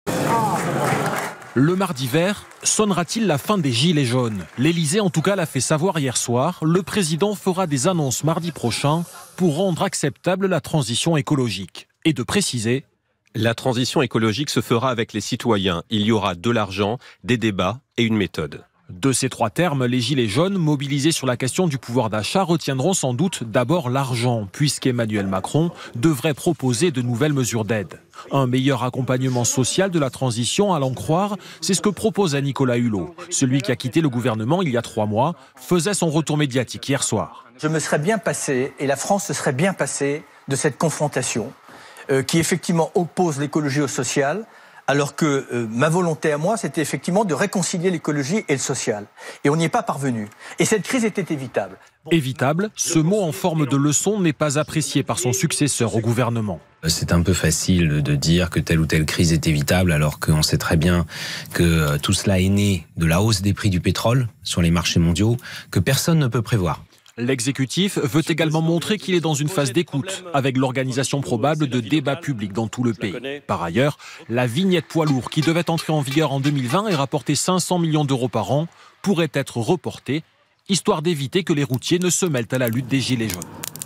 Gilets jaunes : Emmanuel Macron fera des annonces mardi
Pour réinstaurer le dialogue en pleine crise des "gilets jaunes", l'exécutif chercherait à territorialiser le débat, à le décentraliser et, pourquoi pas, à organiser des rencontres en régions.